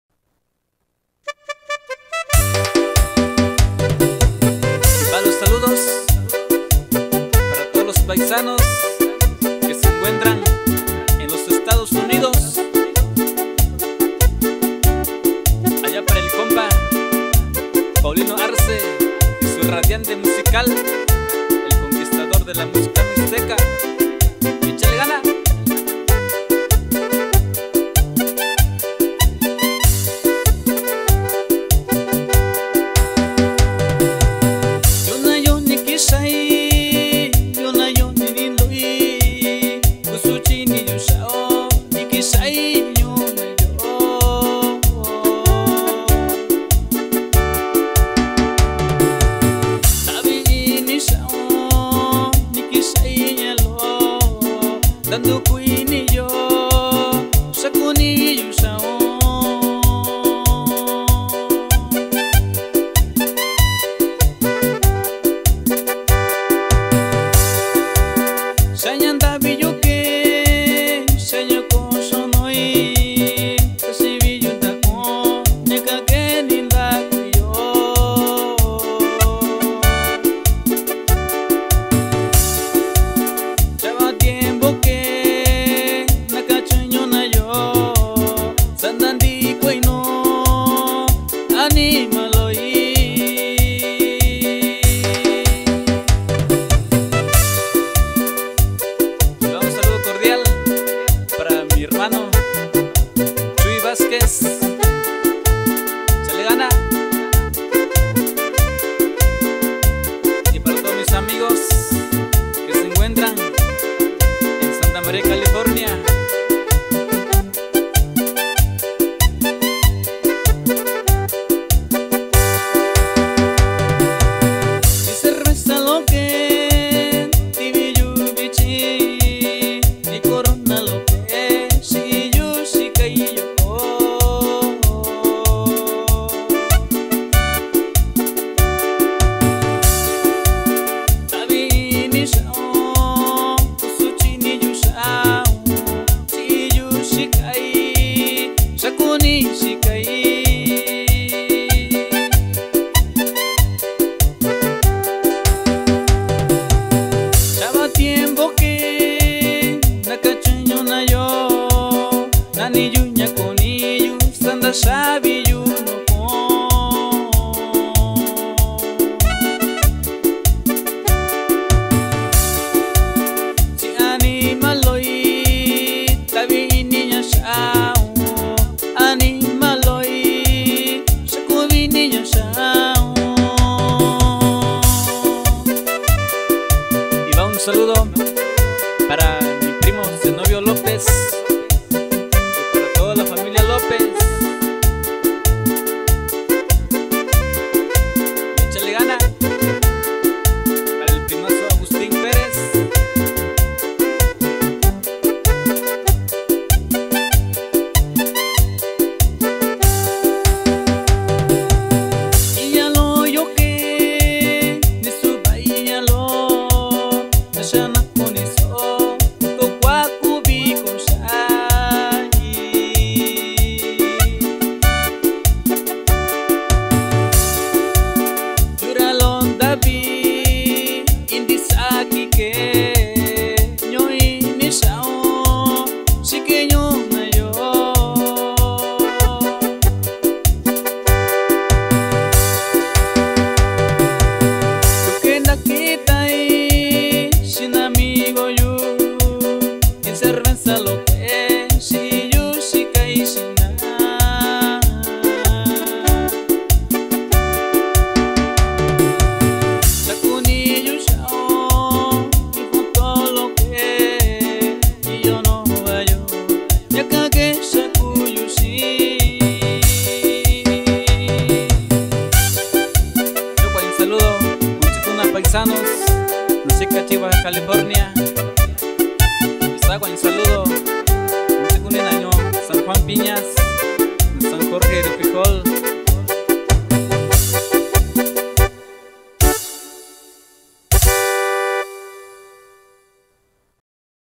Musica Mixteca-Fortaleza Musical
AQUI LES DEJO UNA MUSICA MIXTECA AL ESTILO DE FORTALEZA MUSICAL LA LEYENDA DE LA MUSICA MIXTECA DE SAN JORGE RIO FRIJOL, OAXACA.